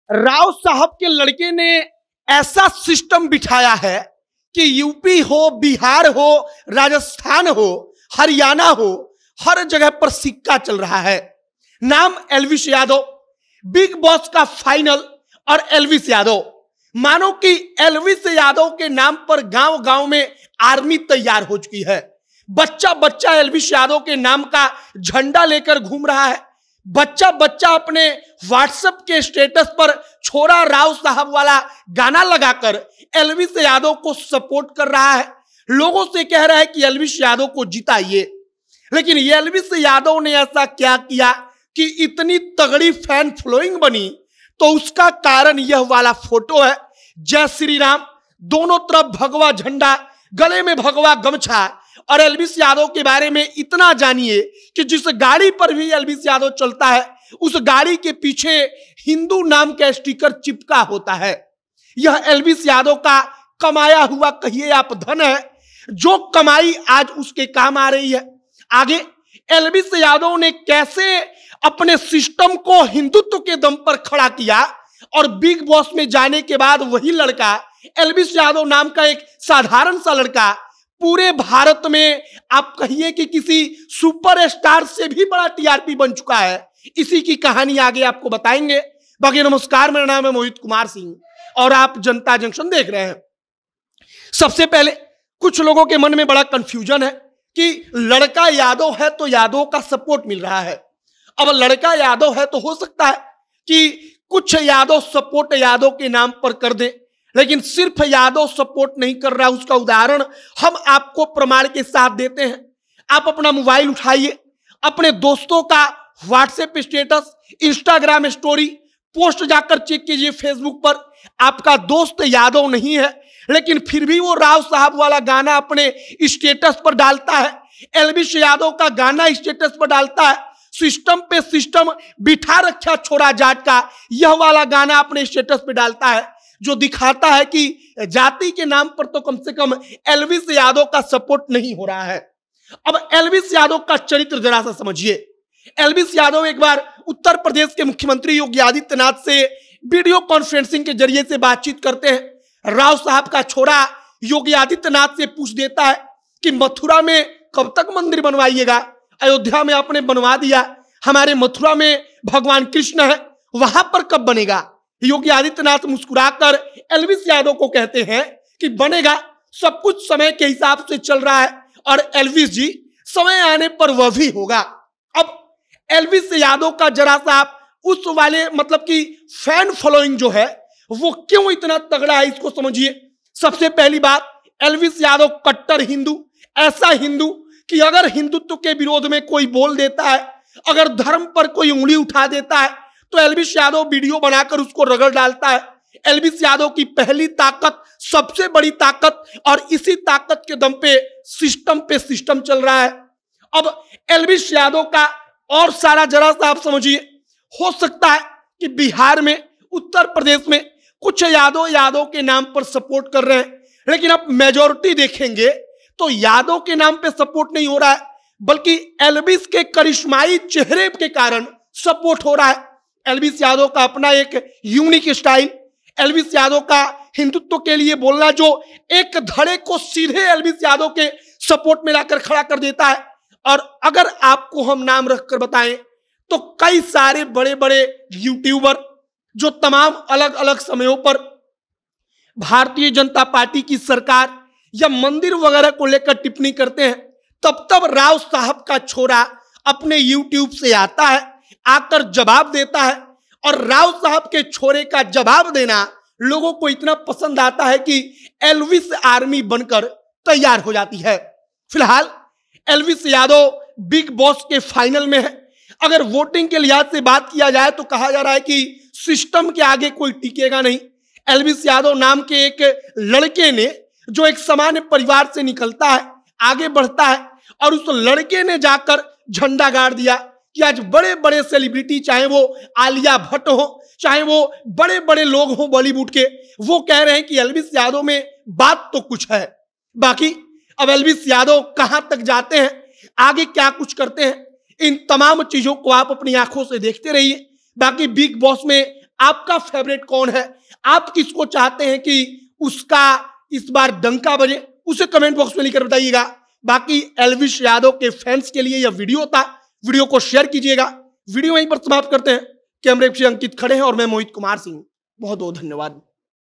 Elvish Yadav एक लड़का जो Hindu बनकर झंडा गाड़ रहा है, Salman Khan के स्टारडम को चैलेंज कर रहा!
एलविश यादव से सीखिए, लड़का यादव का है लेकिन उसका स्टेटस राजपूत, ब्राह्मण, भूमिहार, गुर्जर सब लगा रहे हैं! योगी और मोदी का चेला है राव साहब का छोरा!